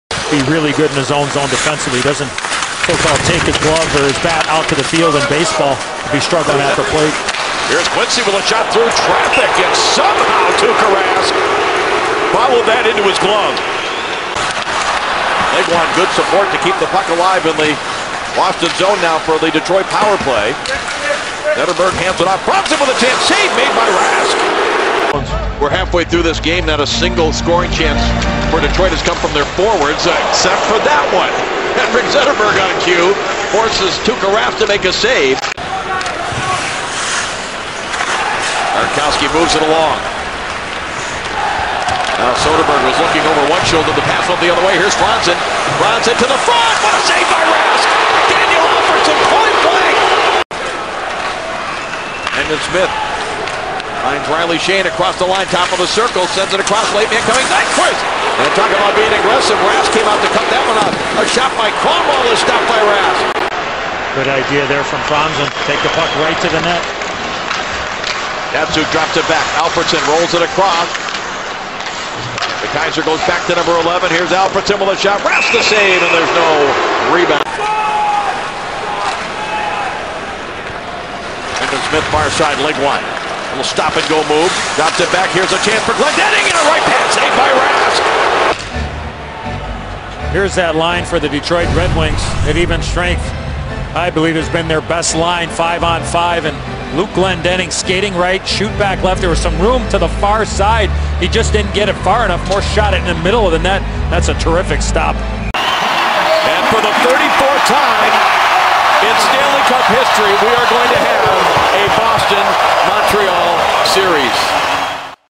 Tuukka Rask best saves of the game vs Red Wings
The Black & Gold defeat the Red Wings 4-2 to take the opening round series in five games and set up a matchup with the Habs.